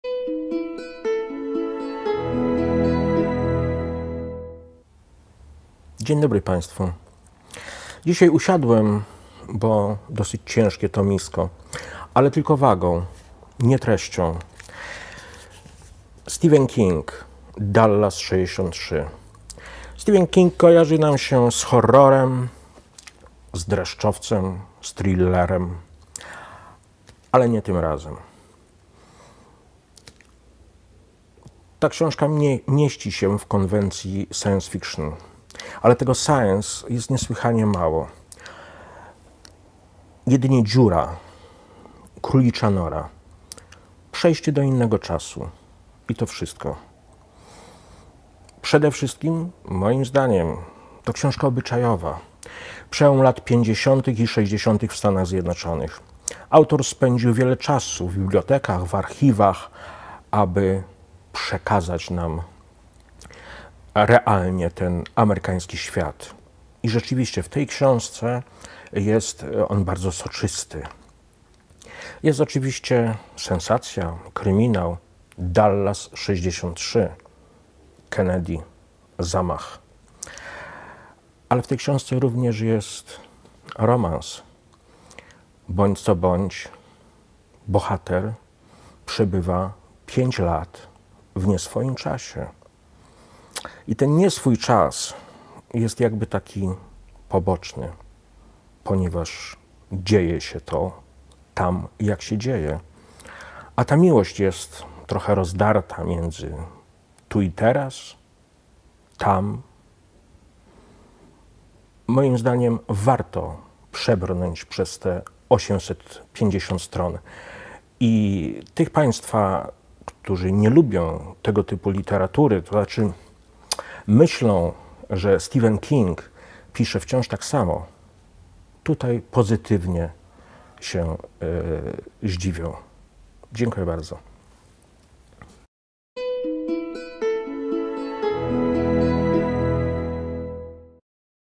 Polana lektury 02
Subiektywna recenzja, zachęta do przeczytania książki „Dallas 63” Stephena Kinga, oczywiście publikacja dostępna w Bibliotece Polany Kultury.